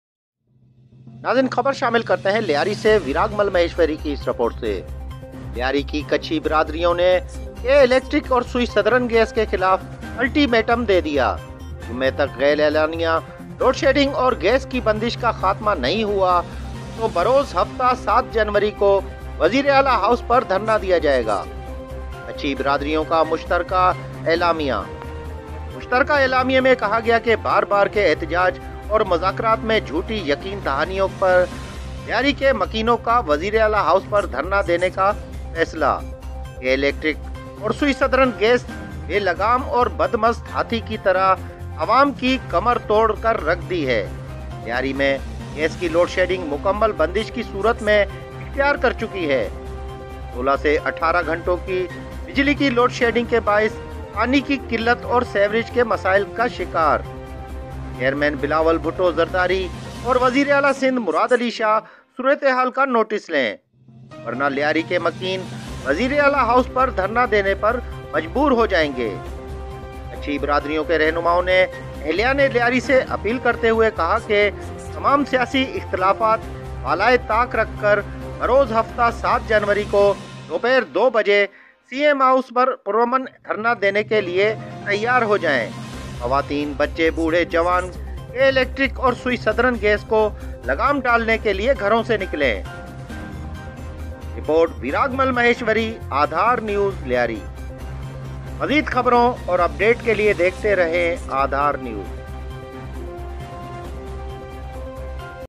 لیاری کی کچھی برادریوں نے کے الیکٹرک اور سوئی سدرن گیس کیخلاف الٹی میٹم دے دیا
رپورٹ پیش کرتے ہین لیاری سے  ویراگ مل مھیشوری کی۔۔۔۔۔۔

*جمعہ تک غیر اعلانیہ لوڈشیڈنگ اور گیس کی بندش کا خاتمہ نہیں ہوا تو  بروز ہفتہ 7 جنوری کو وزیر اعلی ہاؤس پر دھرنا دیا جائیگا
کچھی برادریوں کا مشترکہ اعلامیہ
مشترکہ اعلامیہ میں کہا گیا ہے کہ بار بار کے احتجاج اور مذاکرات میں جھوٹی یقین دہانیوں کے بعد لیاری کے مکینوں کا وزیر اعلی ہاؤس پر دھرنا دینے کا فیصلہ
 کے الیکٹرک اور سوئی سدرن گیس بے لگام اور بدمست ہاتھی کی طرح عوام کی کمر توڑ کر رکھ دی ہے
لیاری میں گیس کی لوڈ شیڈنگ مکمل بندش کی صورت اختیار کر چکی ہے
*16 سے 18 گھنٹوں کی  بجلی کی لوڈ شڈنگ کے باعث پانی کی قلت اور سیوریج کے مسائل کا شکار
 *چیئرمین بلاول بھٹو زرداری  اور وزیر اعلی سندھ مراد علی شاھ صورتحال کا نوٹس لیں
ورنہ لیاری کے مکین   وزیر اعلی ہاؤس پر دھرنا دینے پر مجبور ہو جائیں گے
کچھی برادریوں کے رھنماؤوں نے اہلیان لیاری  سے اپیل کرتے ہوئے کہا ہے کہ تمام سیاسی اختلافات بالائے طاق رکھ کر بروز ہفتہ 7 جنوری کو دوپہر دو بجے  سی ایم ہاؤس پر پرامن دھرنا دینے کے لئے تیار ہوجائیں خواتین بچے بوڑھے جوان کے الیکٹرک اور سوئی سدرن گیس کو لگام ڈالنے کے لئے گھروں سے نکلیں